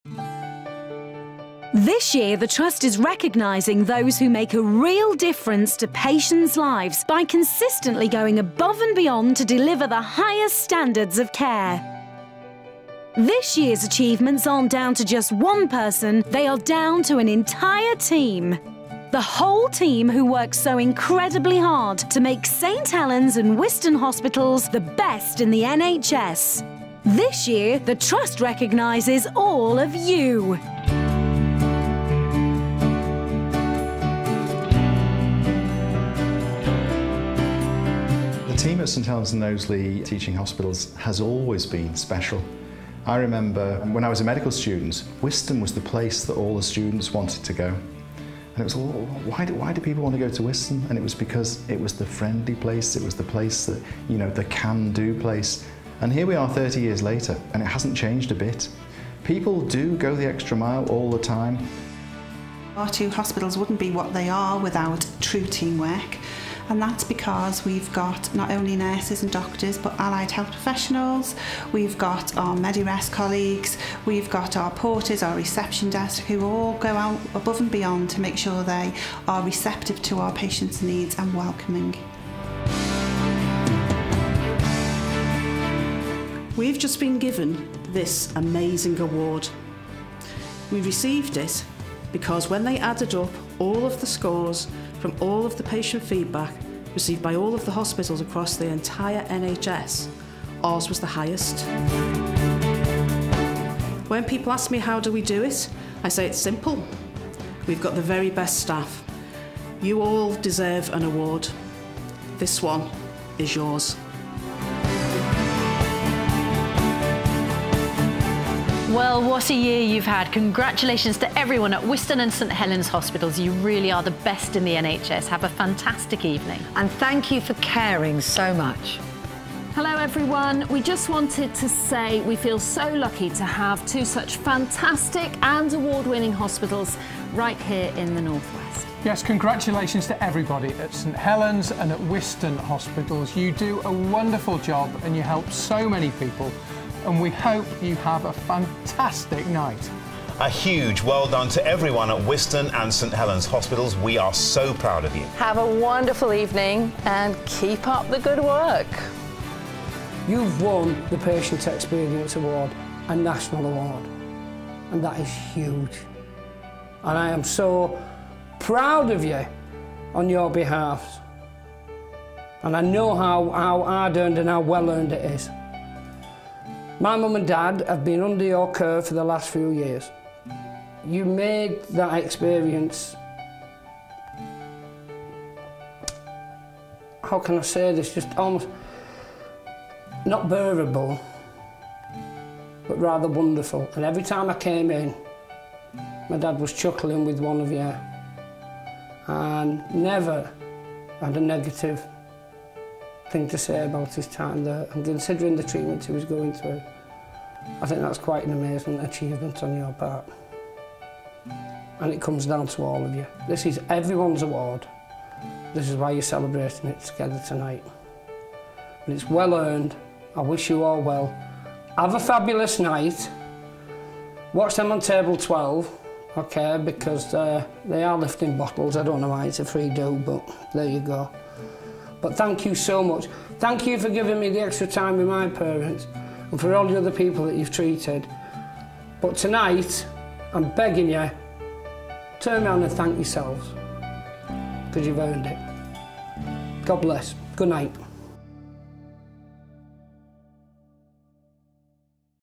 Special Recognition Award 2015
We are delighted to have won the ‘Best Patient Experience in the NHS’ at the CHKS Top Hospitals Awards 2015.
To recognise the continued hard work and professionalism of everyone at the Trust, all our staff received a special recognition at this year’s Annual Staff Awards 2015.